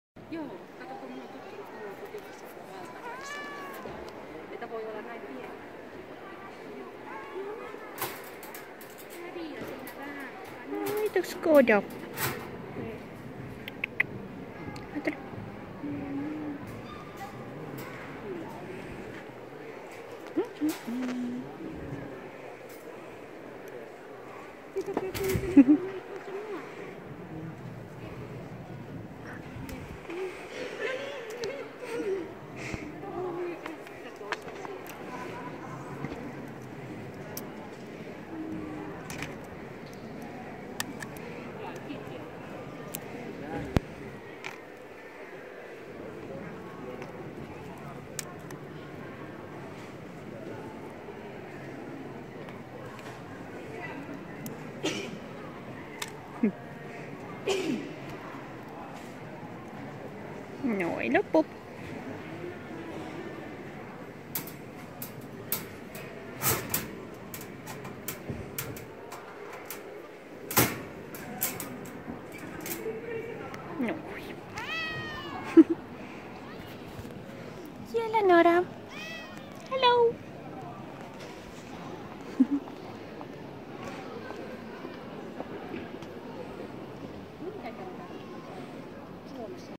Somali cat and american curl at a cat show
Koda (somali cat, 1 y) and Nora (long hair american curl, 7 y) in their show cages. Nora is always so relaxed and calm at shows, meanwhile Koda loves the attention and wants action. :D